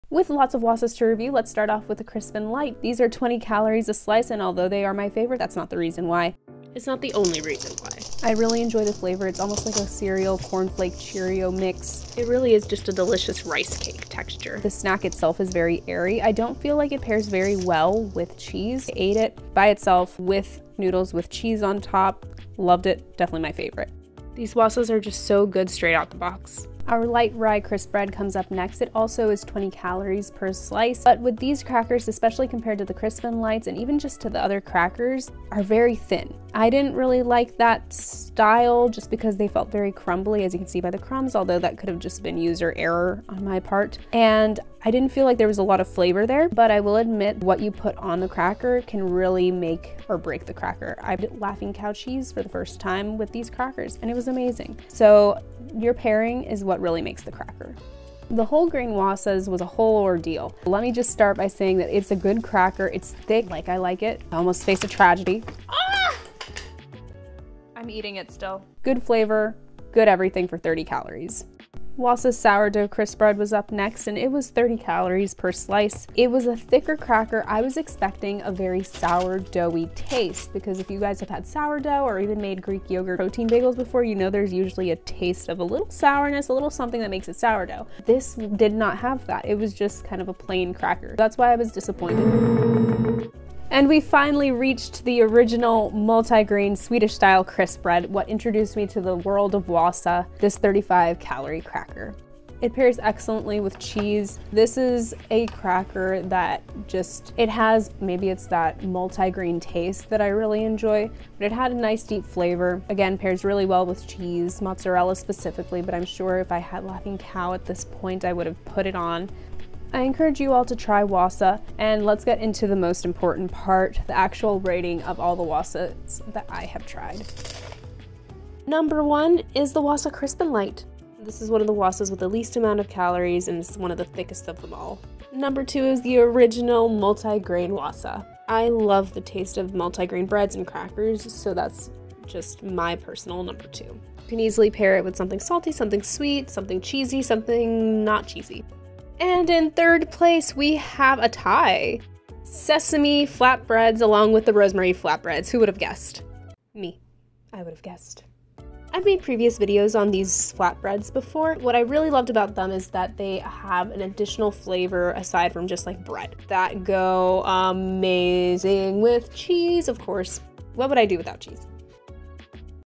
THE BEST WASA CRACKER IS....
THE WASA DATA COLLECTION IS COMPLETION
I tried every wasa I could find at Publix. 9 to be completely clear.
I tasted each one and here are the results.